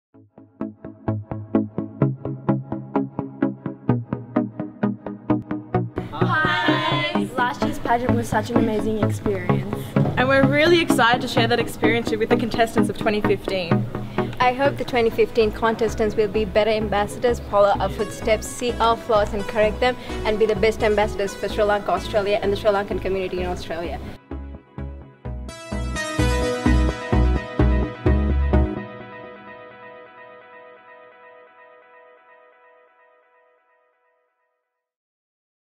Miss Sri Lanka Australia 2015 - last year finalists
2014 finalists sharing their experiences at the workshop for 2015 contestants.